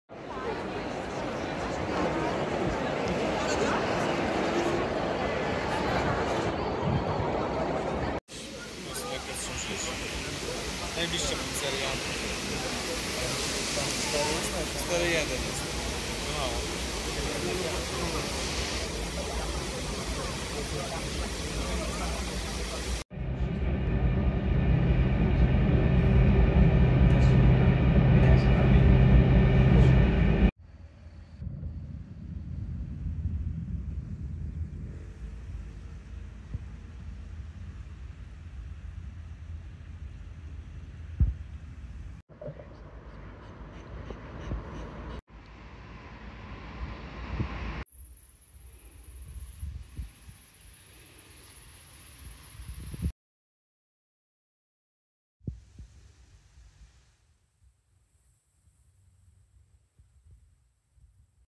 Pixel 8 pro - film testowy
google Pixel8Pro to najnowszy flagowy smartfon od Google, który kusi 7-letnim wsparciem oprogramowania, procesorem Tensor G3 z obsługą 5G, czystym Androidem 14 z funkcjami AI, wyświetlaczem OLED 120 Hz i imponującym aparatem.
Czy ten smartfon warty jest swojej ceny? Sprawdziliśmy to!